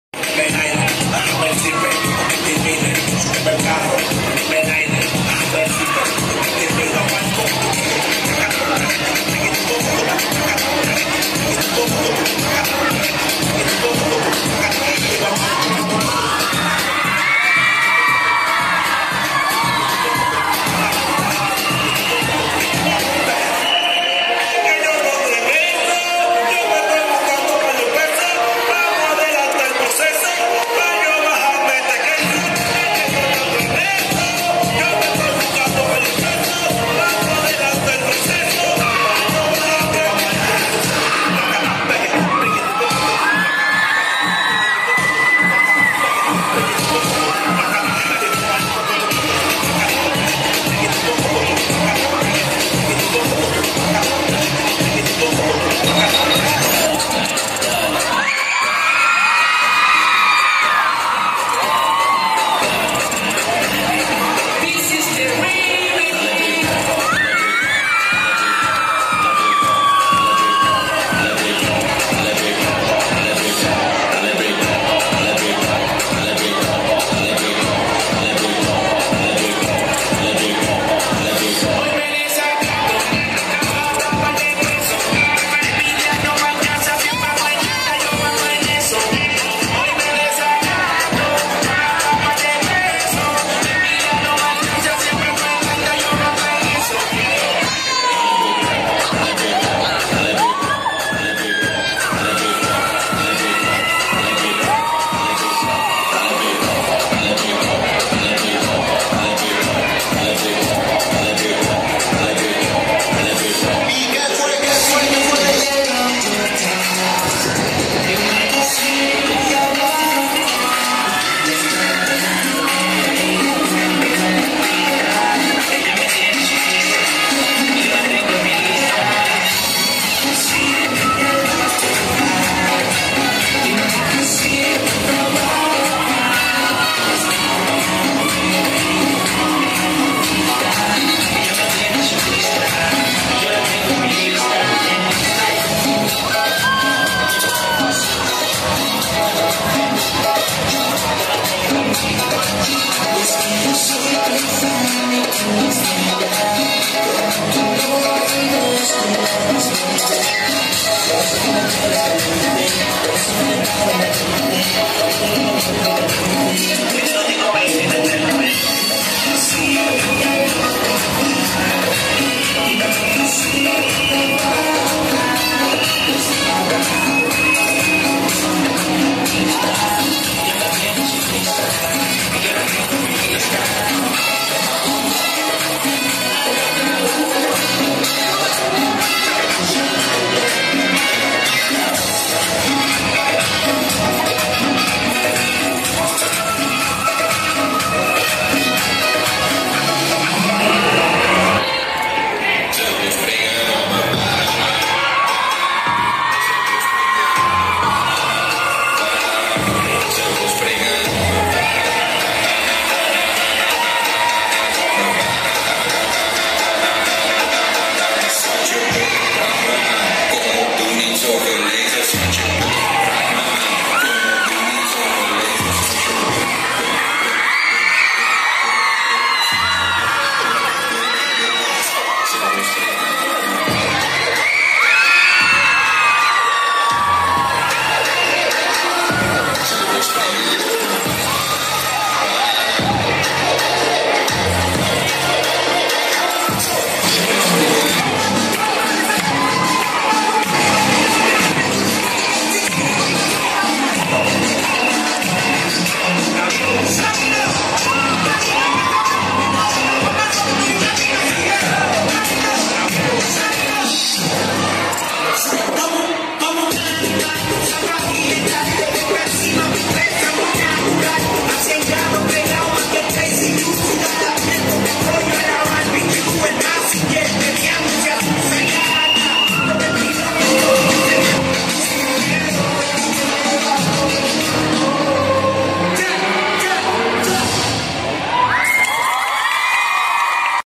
PATRÓN URBANO | Elección de la reina de carnaval 2019 Colegio Coromoto
PATRÓN URBANO

Grupo de baile de Calabozo - Venezuela